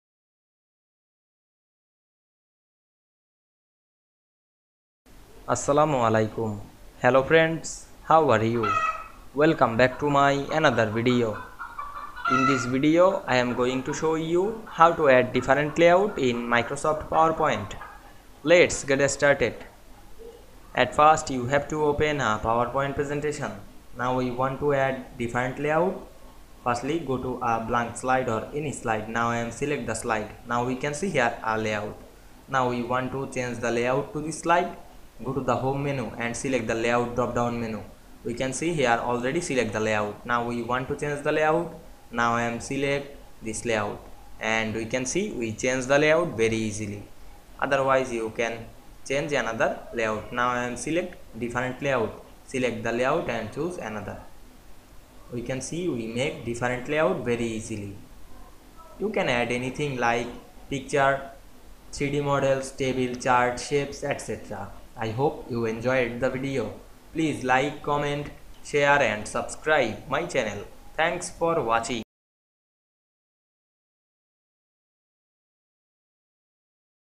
How to add different layout in PowerPoint
Assalamu Walaikum,
In this video I will show you, How to add different layout in PowerPoint.  Let's get started.